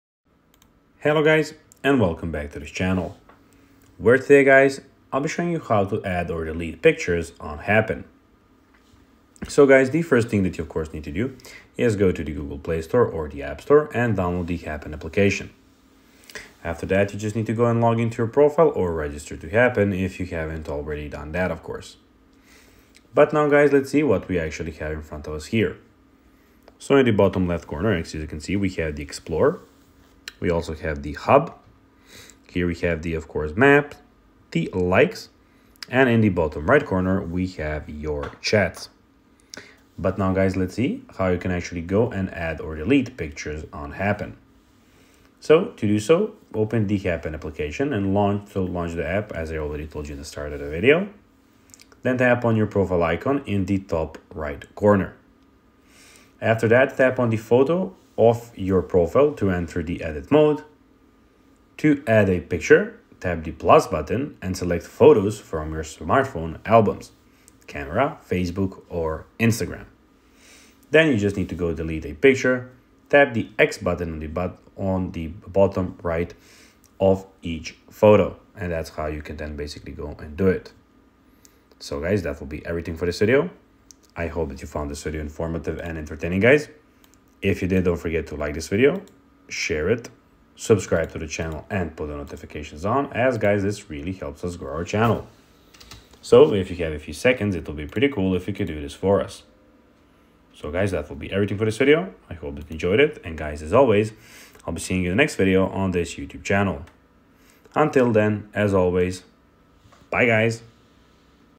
How To Add Or Delete Pictures On Happn (Easiest Way)​​​​​​​
In this video I will show you How To Add Or Delete Pictures On Happn. It would be good if you watch the video until the end so that you don't miss important steps.